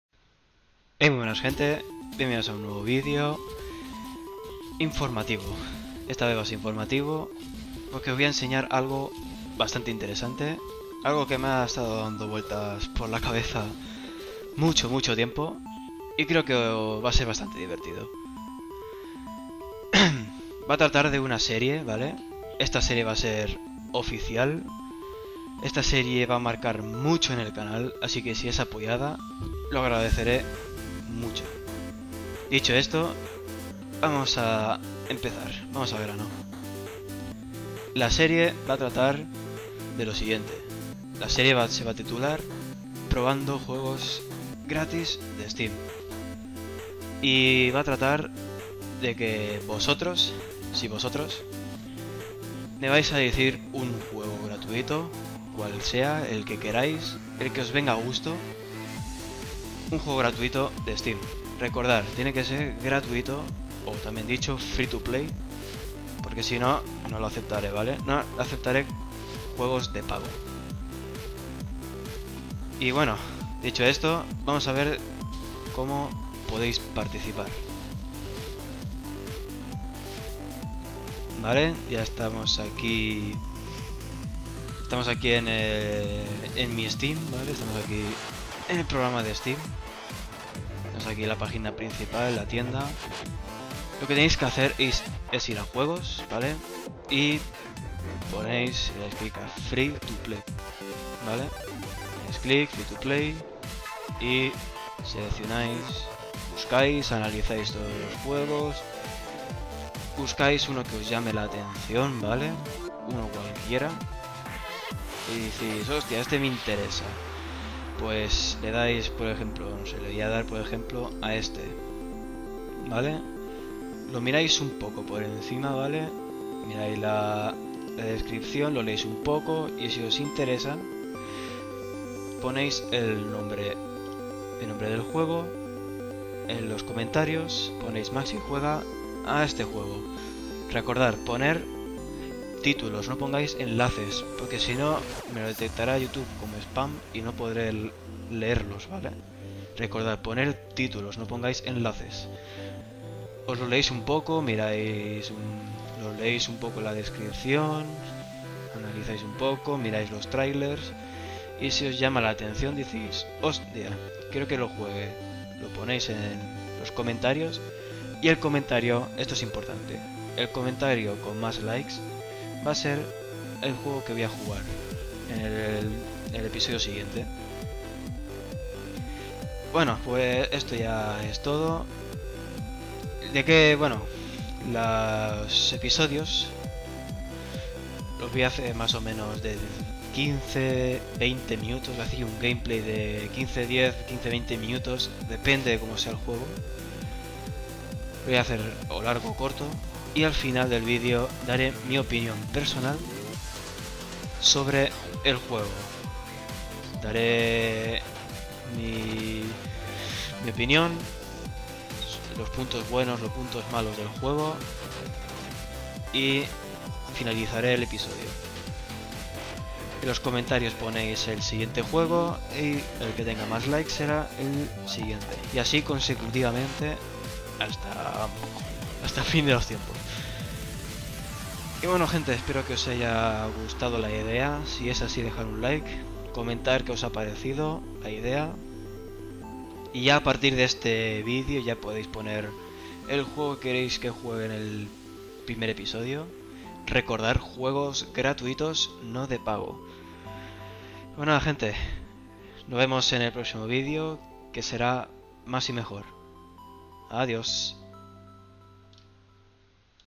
Vídeo informativo: Nueva serie OFICIAL del canal
Buenas gente, en éste vídeo os explico detalladamente de que va a tratar la nueva serie del canal, espero que os guste la idea y que le deis un apoyo enorme. :D